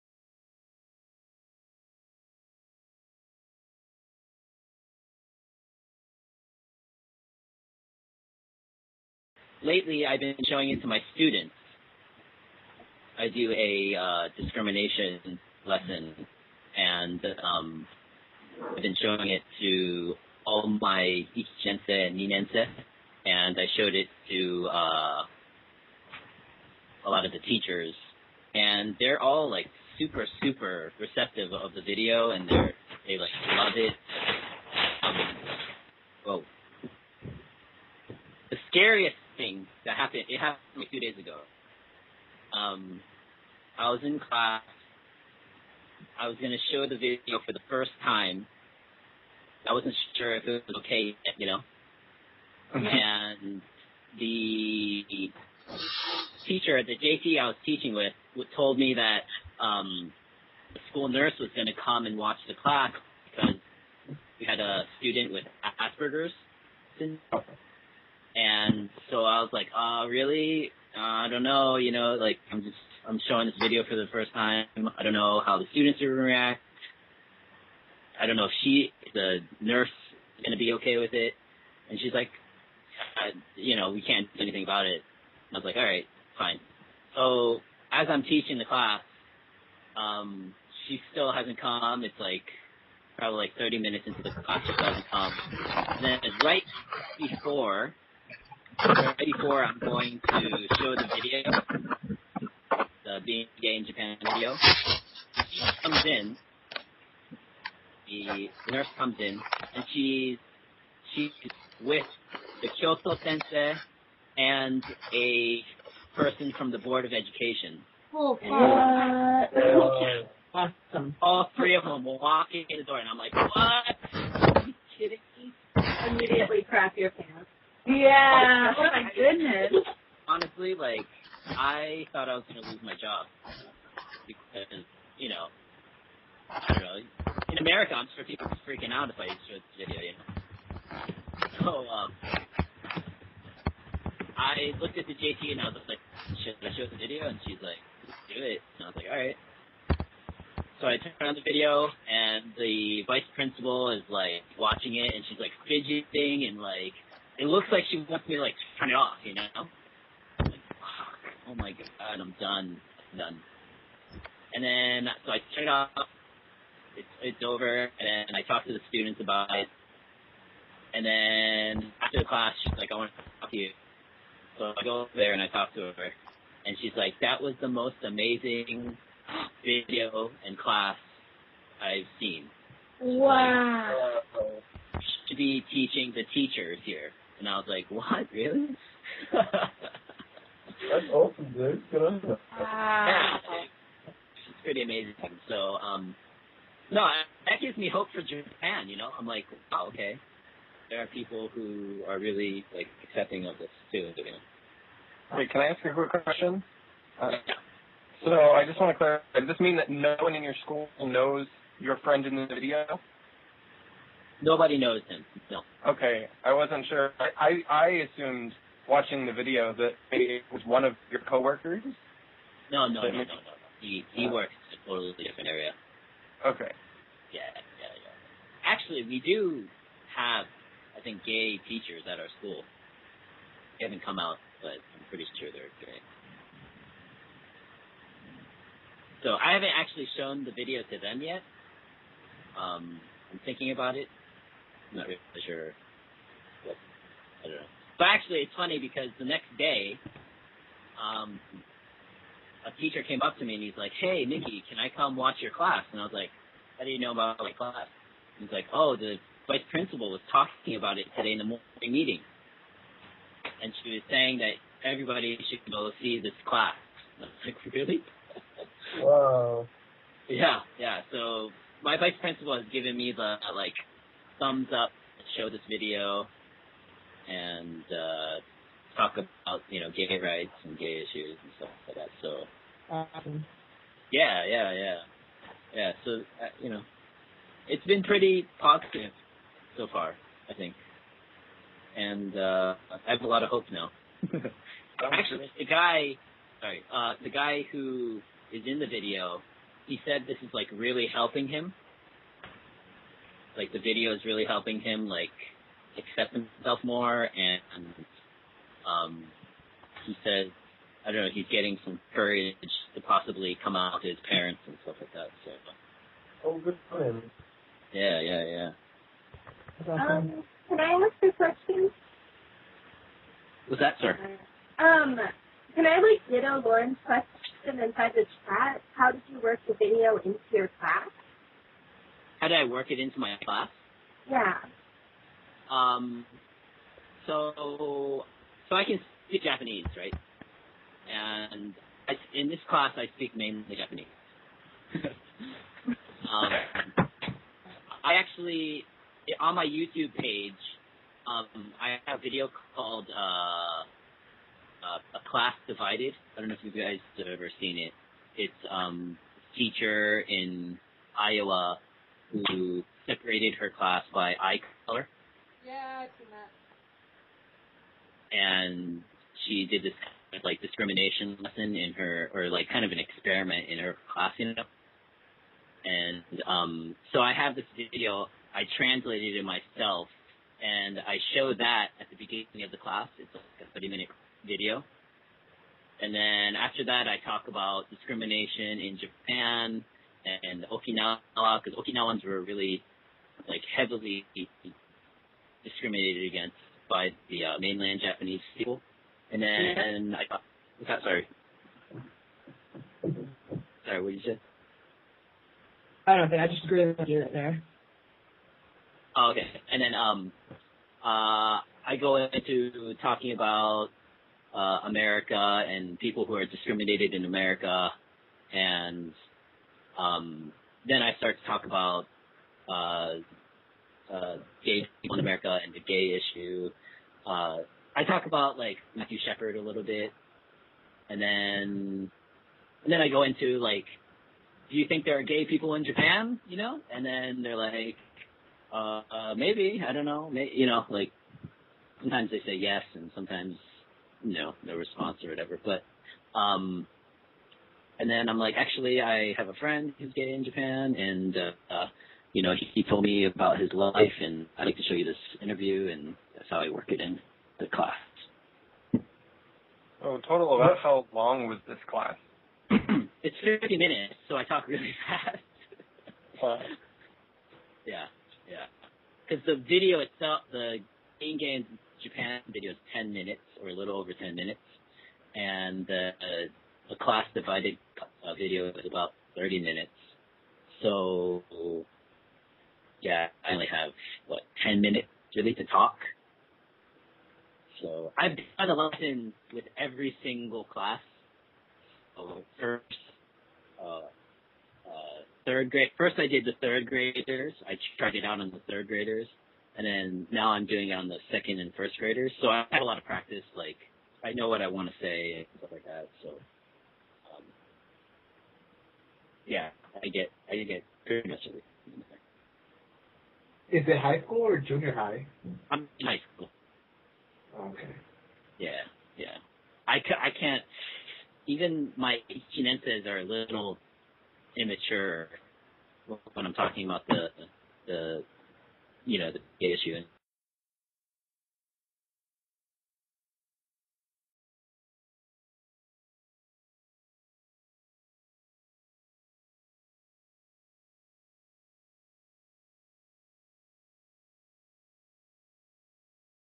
Skype Chat Interview with Medama Sensei's Video "Being Gay in Japan" PART 2
On June 30th, 2012, Medama Sensei posted a video on Being Gay in Japan, which has currently over 30,000 hits. On July 5th, Stonewall AJET hosted a Skype Chat interview with the producer of the video to learn about these issues.

Find out about LGBT events in Japan by joining our Facebook group and following our Twitter account @stonewalljapan

Please leave us your comments and "like" the video.